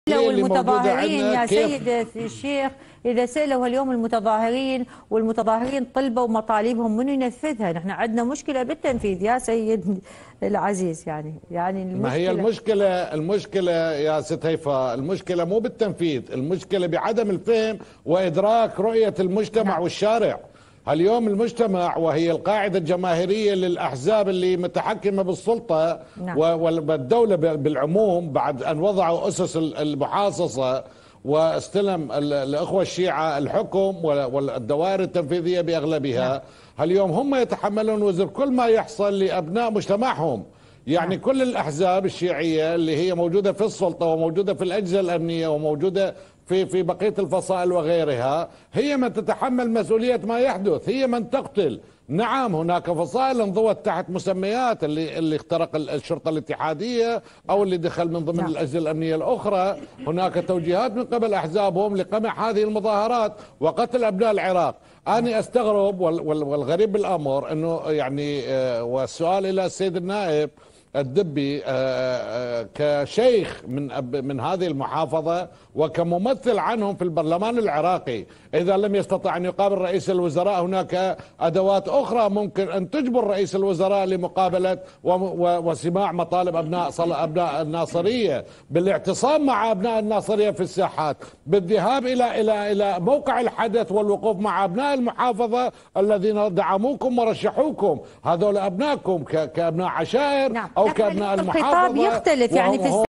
هيفاء الحسيني, الشيخ ثائر البياتي : هناك فصائل مسلحة اخترقت الأجهزة الأمنية لقمع التظاهرات
بلا أقنعة مع هيفاء الحسيني